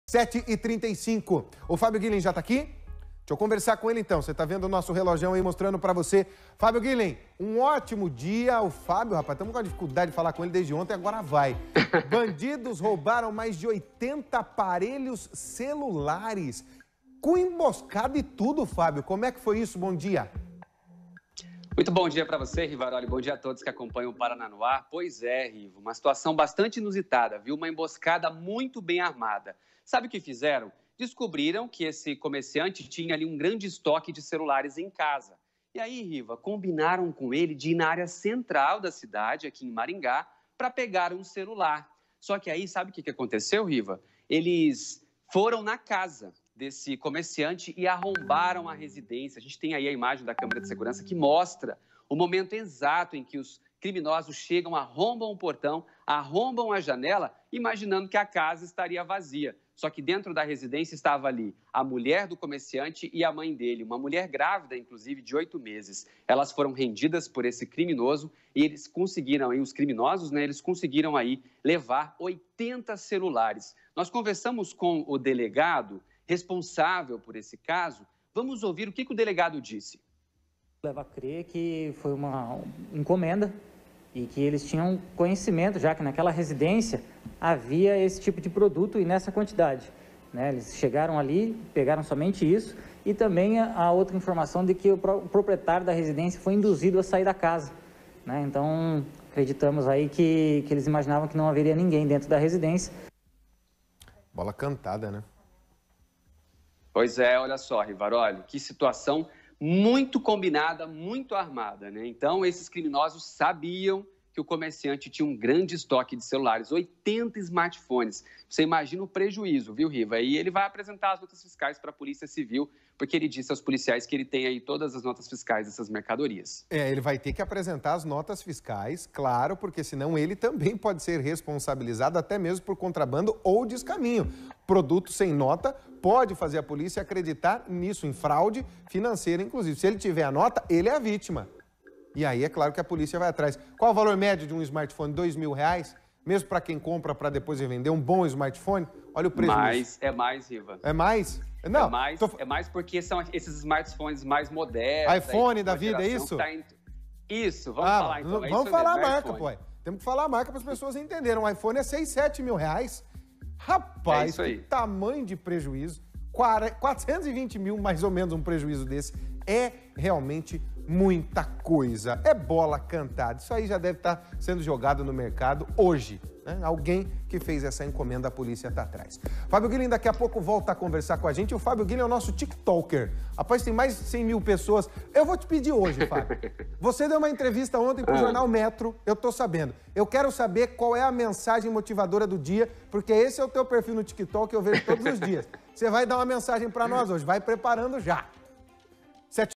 Bandidos armam emboscada para vítima e roubam 80 celulares de uma residência
Bandidos roubaram mais de 80 aparelhos celulares. Os criminosos acreditavam que a casa estava vazia, porém a esposa grávida e a sogra do proprietário dos aparelhos telefônicos estavam na residência e foram rendidas.